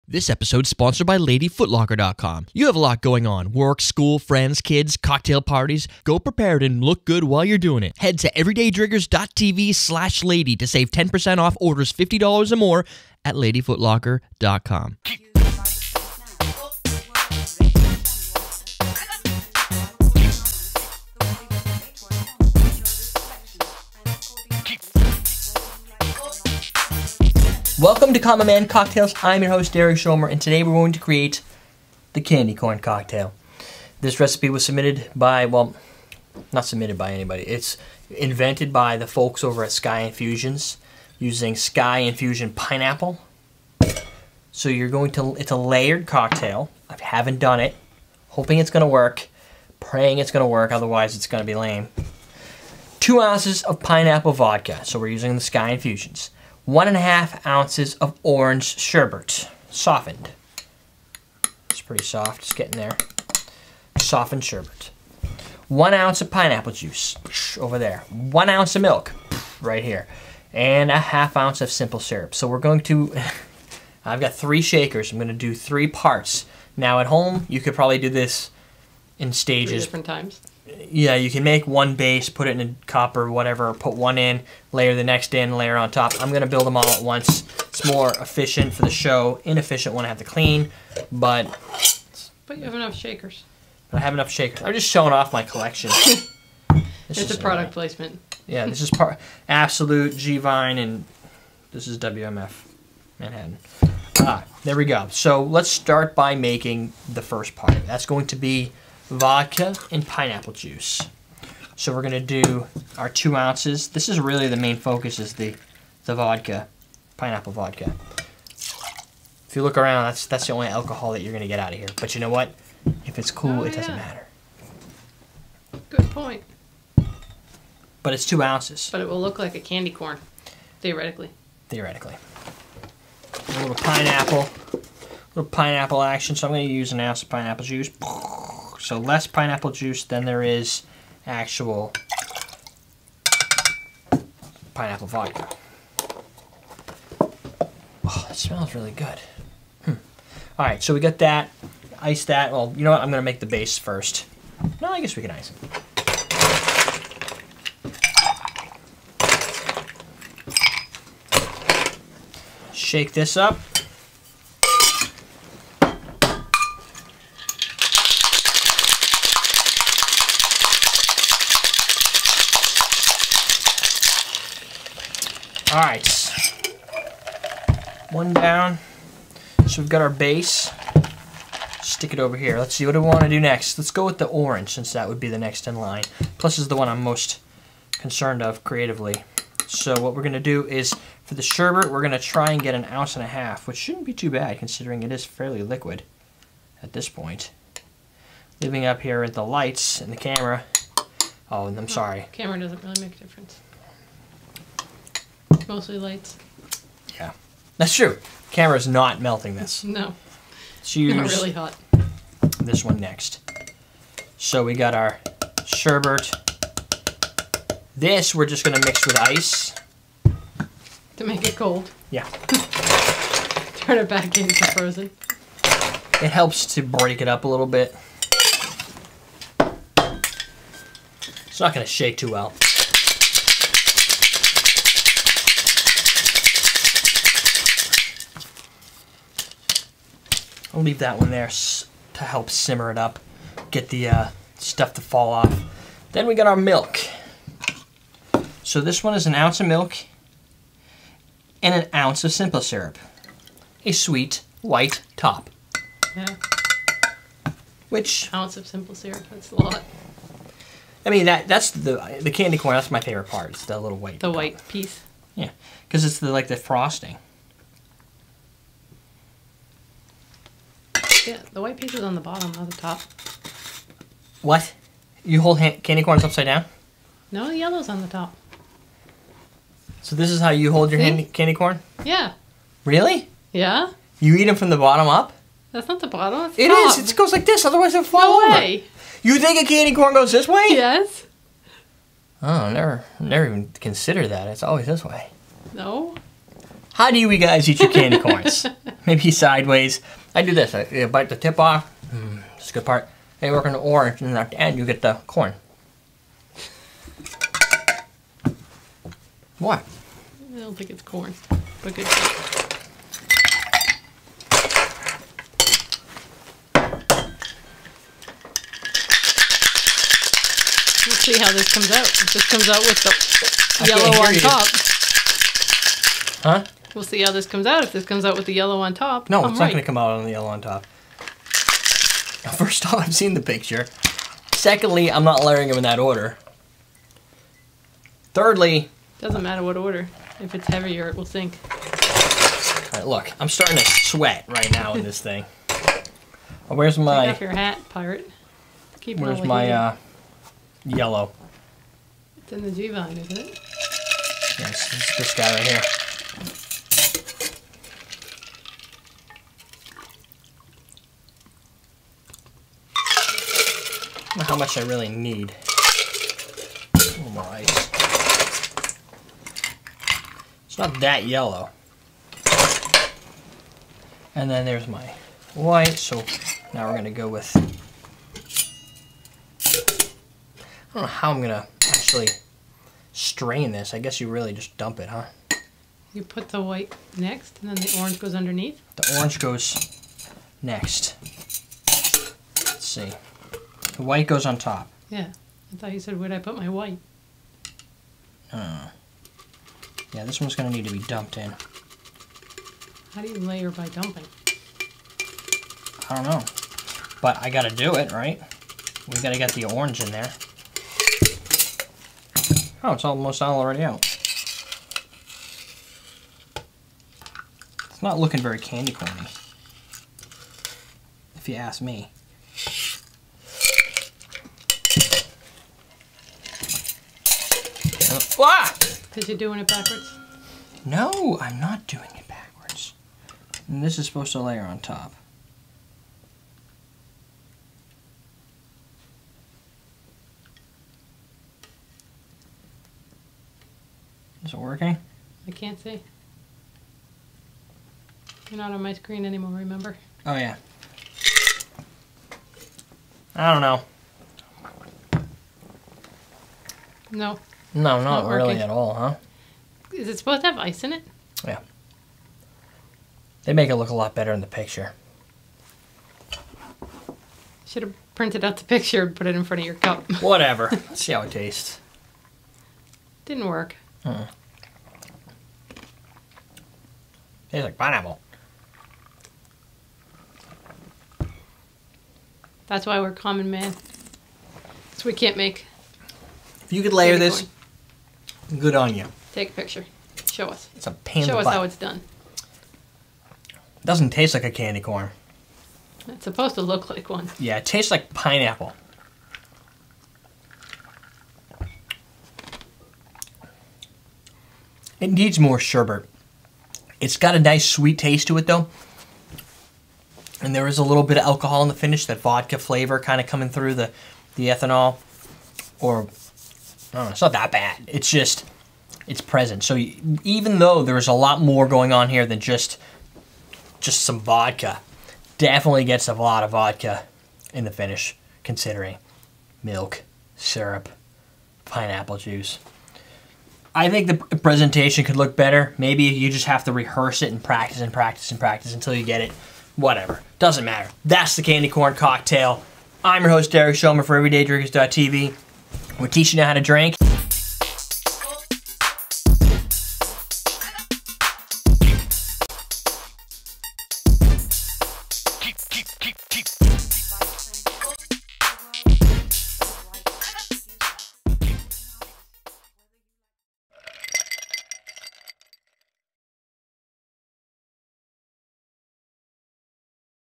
How To Make The Candy Corn Cocktail
Well, this howto video is really howto fail at making the candy corn cocktail in a layered sense, but we did our best. Also, the video is 1-week late due to our power outtage.